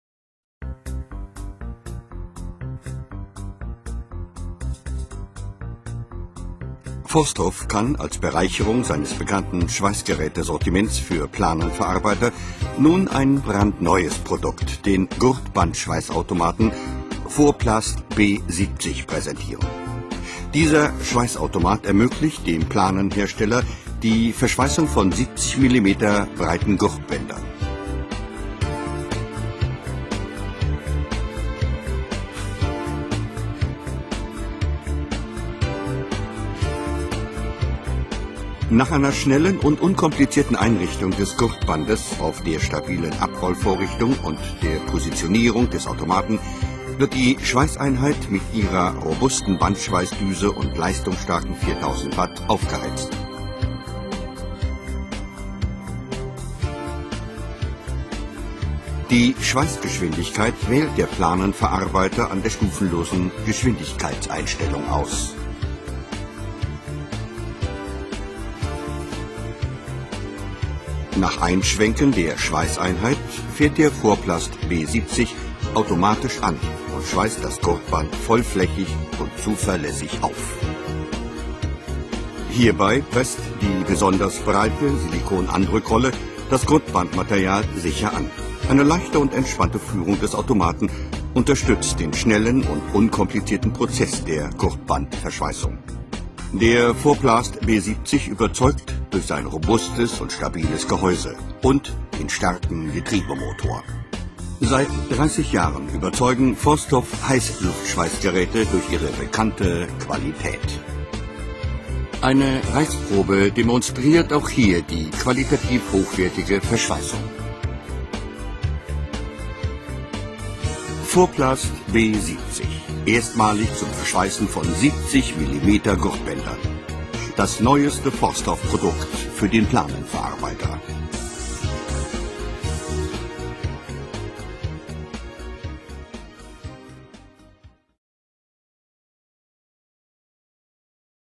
Forplast Hot Air Welder for Webbing Tapes.mp4
Demonstration of the Forplast B 50mm Tape Weld or Webbing Machine.
Presented by Barnes Plastic Welding Equipment Limited.